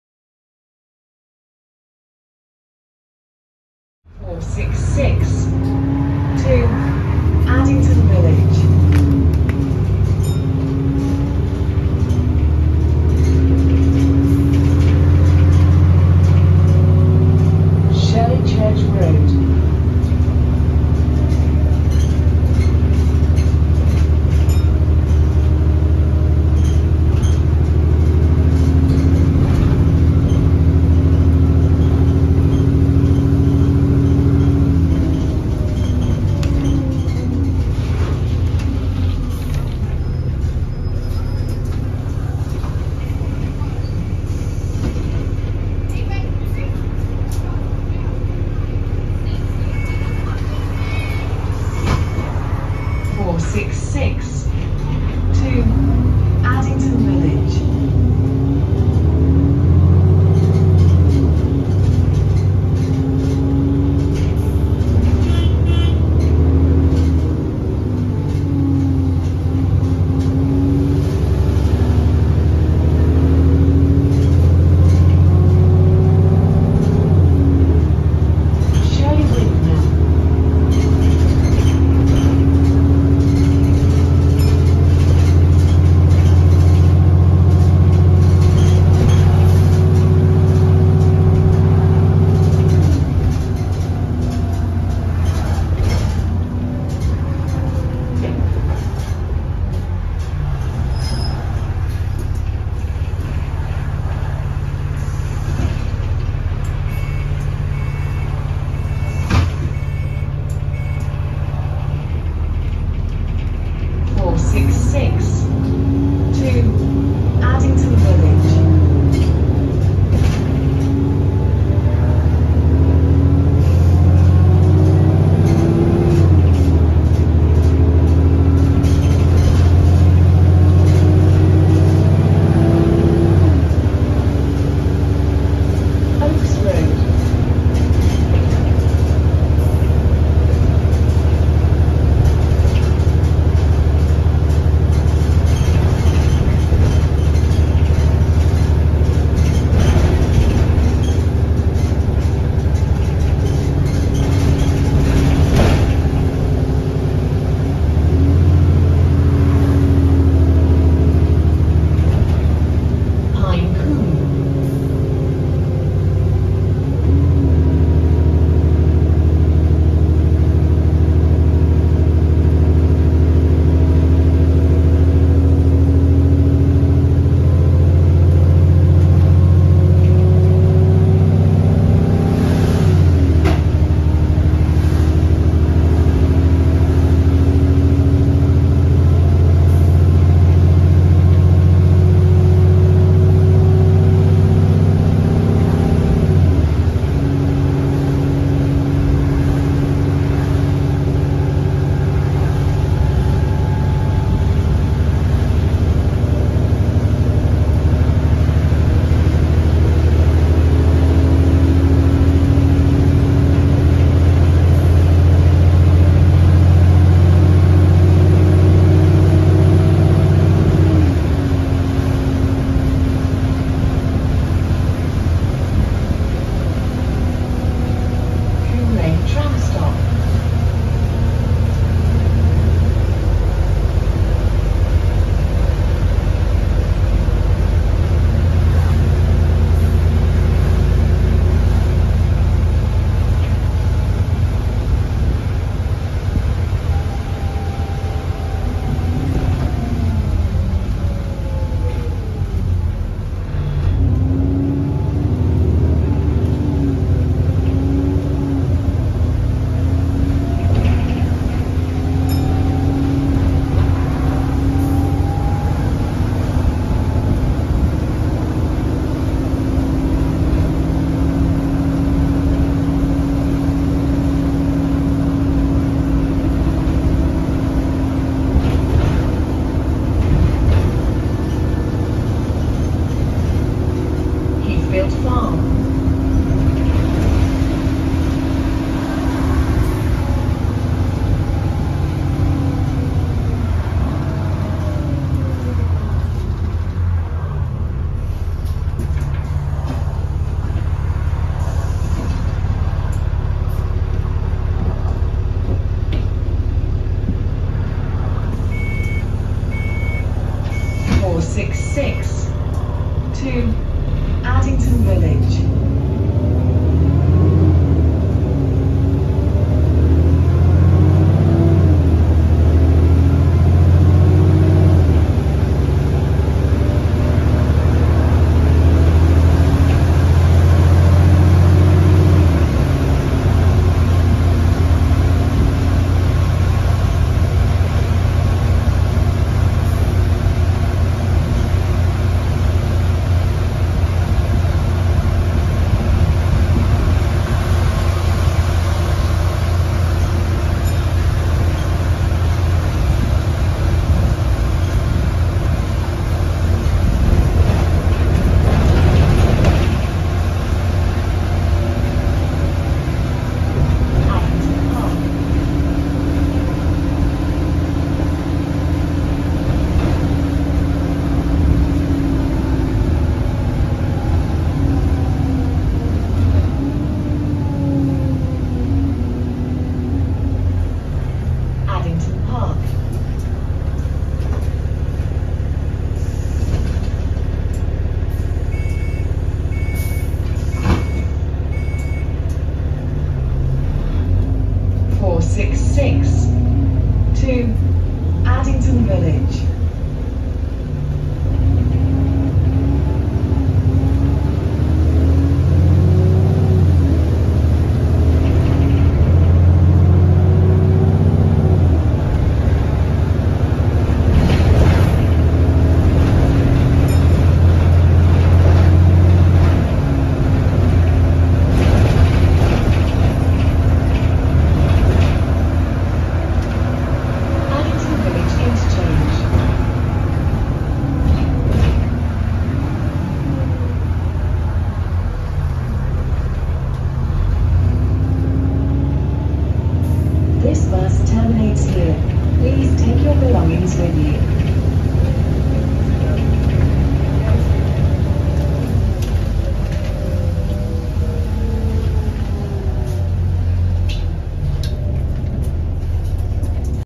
*OVERREVS* Journey on Arriva London South Route 466 - T286 (LJ13 CGK) - Enviro 400 E40D
While going to visual Route 64 this driver was doing a great job driving this bus so I decided to get a journey on it